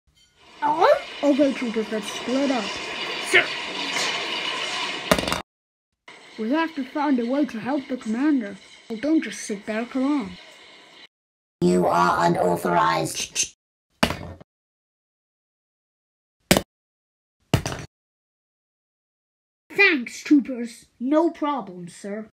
The 104th Episode 3- Saving Wolffe
After defeating B2’s The 104th meet another batch of clankers and it’s up to two clones to save wolffe from them!